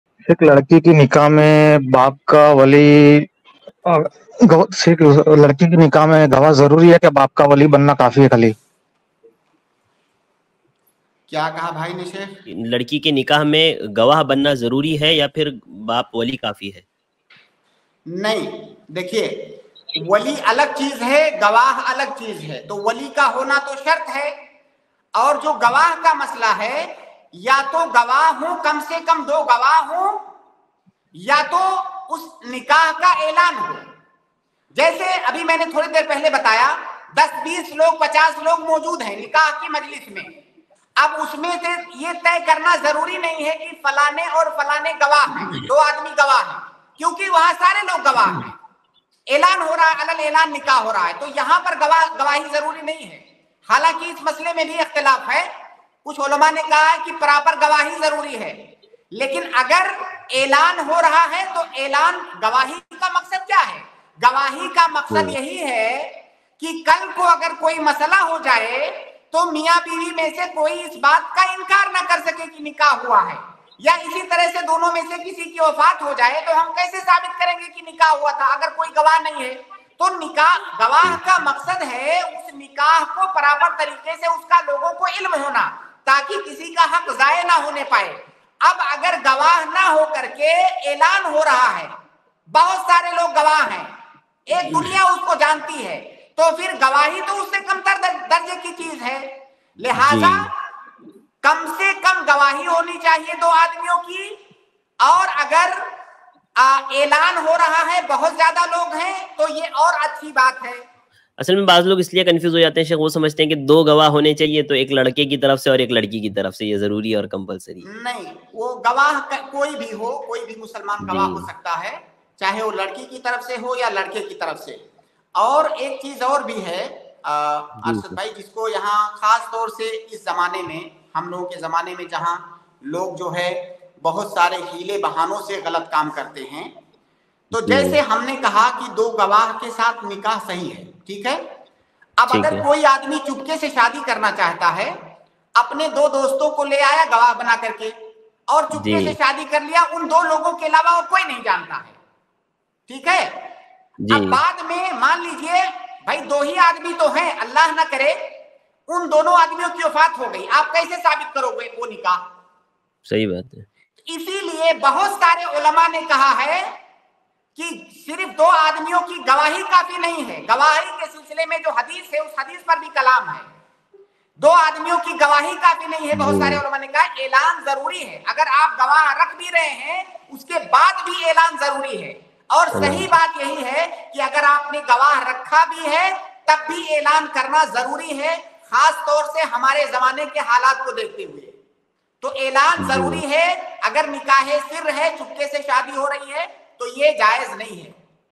Kya Ladki Ke Nikah Mein Gawah Zaruri Hai? By Dr. Mohammad Naseem Madani iPlus TV Deeni Q&A
UPI : donateilm@icici